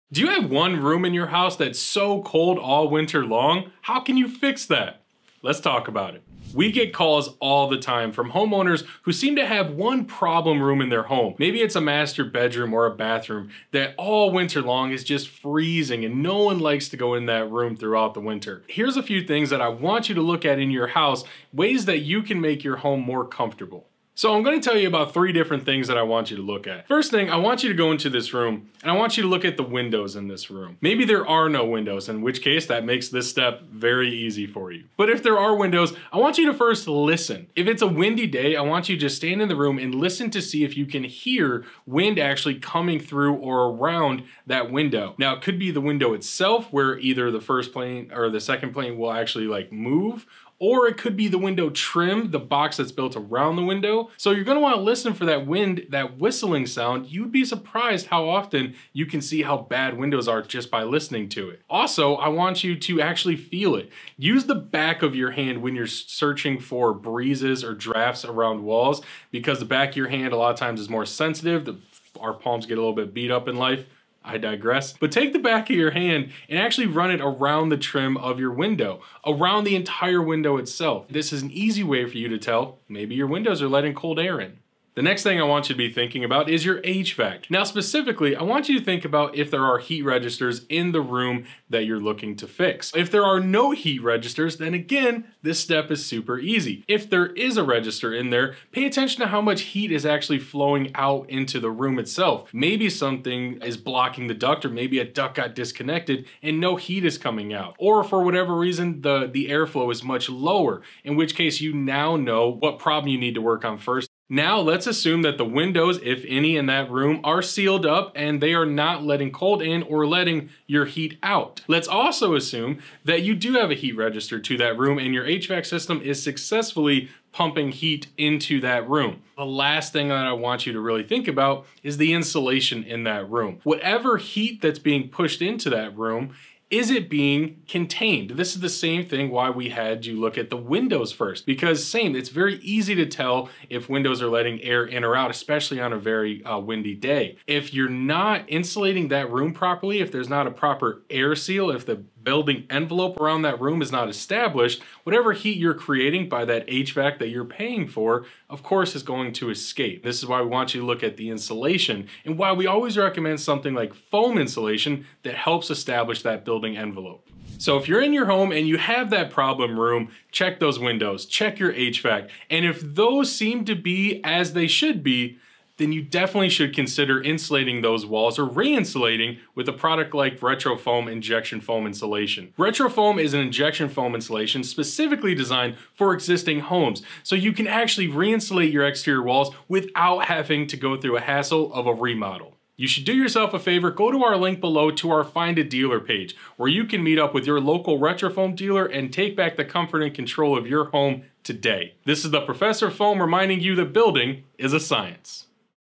Why is My Room So Cold? How to Keep Your House Warm in the Winter | Foam University by RetroFoam
Are you wondering, "Why is one room in my house so cold?" There are a couple of factors, so here's how to keep your house warm in the winter.

0:00 - Intro
0:29 - Step One: Check for drafts around your windows.
0:38 - Listen for wind coming through the windows.
1:04 - Feel for drafts around your window using the back of your hand.
1:25 - Step Two: Check your HVAC system.
1:40 - Check for heat output or if a duct is blocked.
2:09 - Step Three: Check your insulation.

📚Are you in a reading mood? Check out this article for tips to warm up that cold room